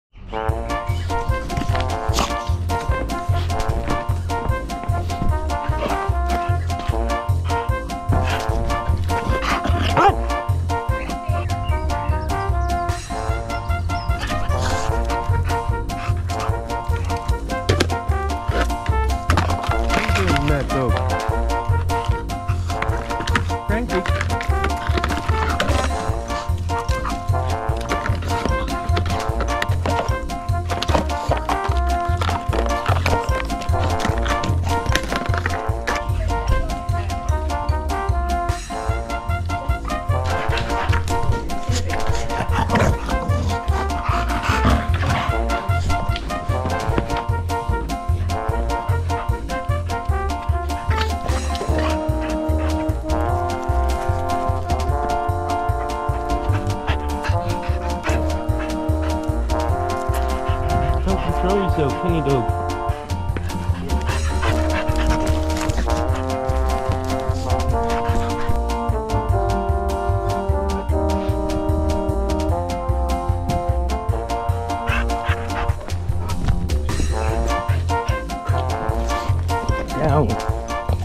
Frankie
This nine year old bulldog needs to chill out.